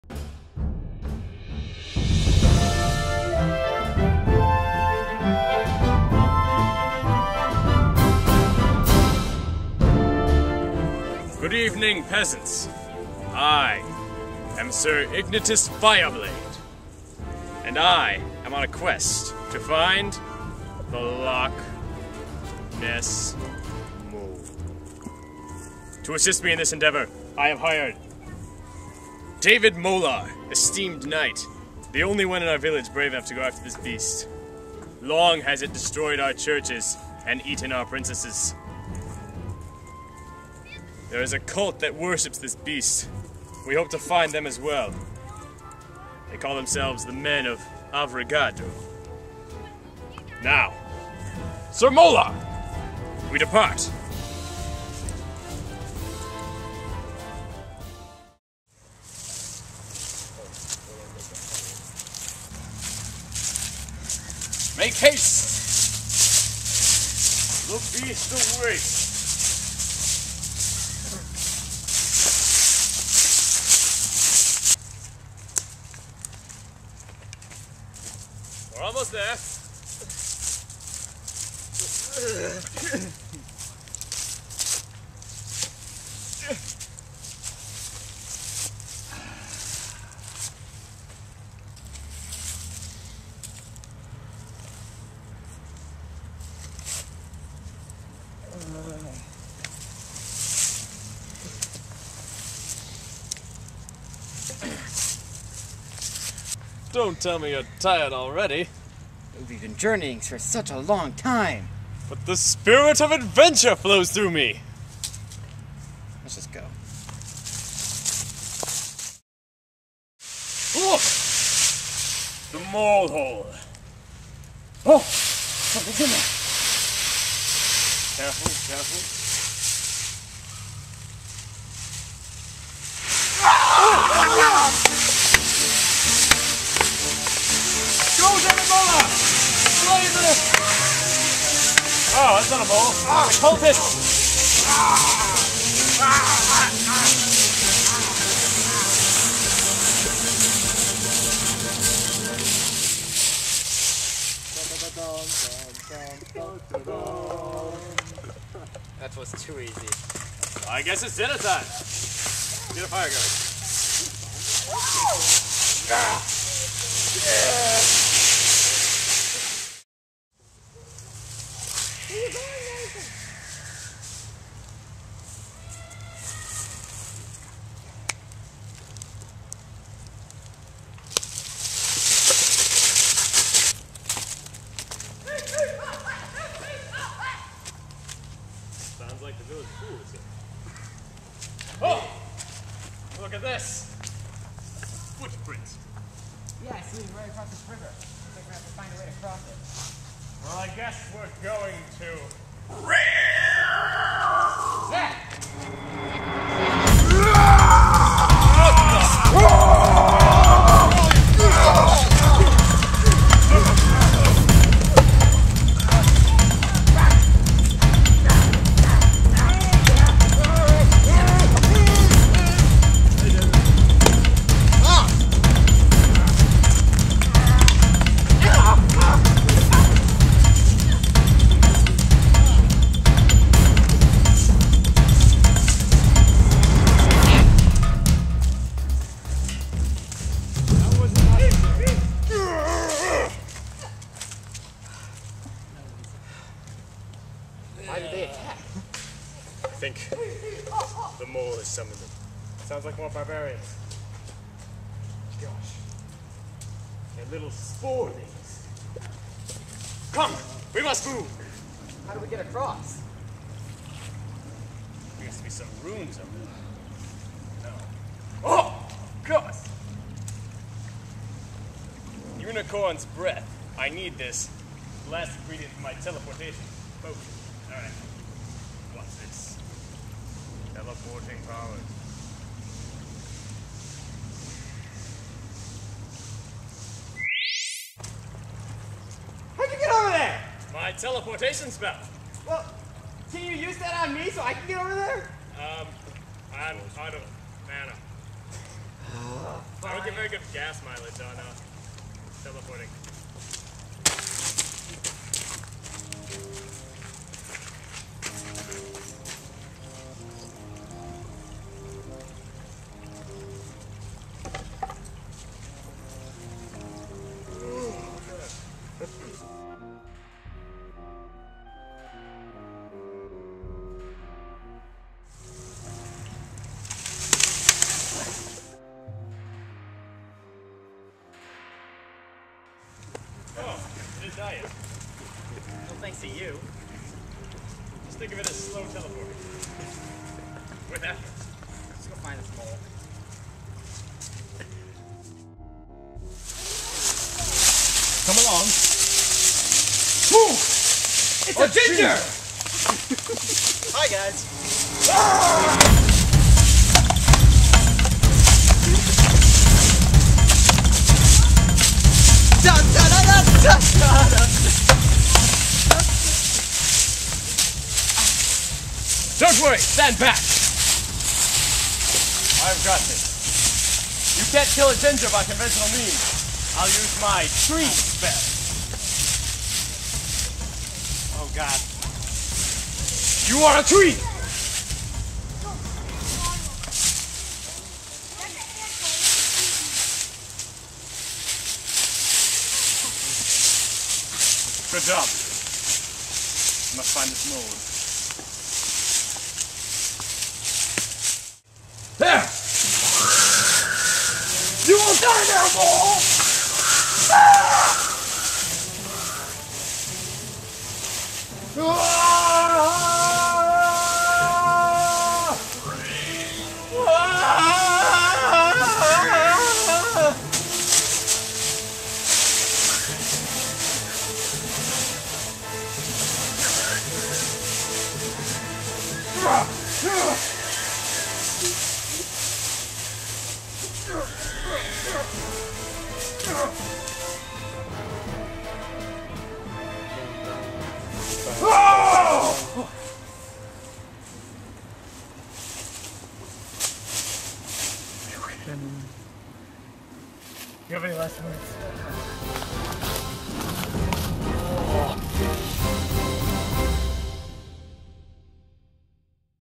The Tale of Sir Ignitus Fireblade
A video project for mole day.  A journey to defeat The Lochness Mole and his Barbarians.